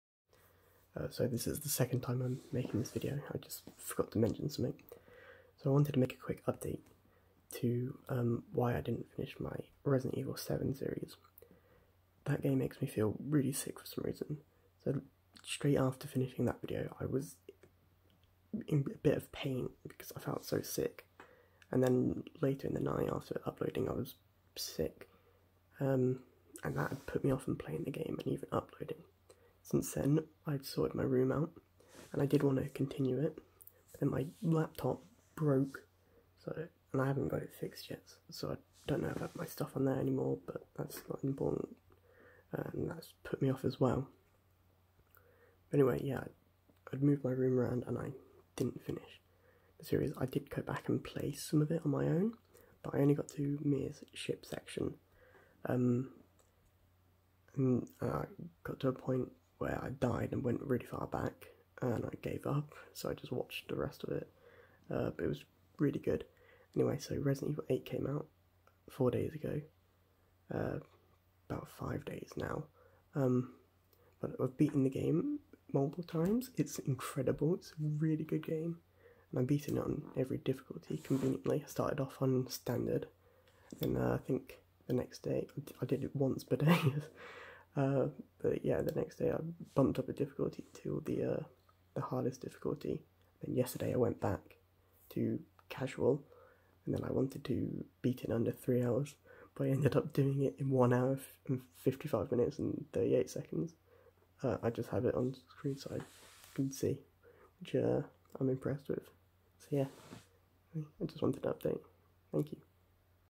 A quick update. + Resident Evil 8 world record?
just me explaining why I stopped my resident evil 7 playthough and talking about resident evil Village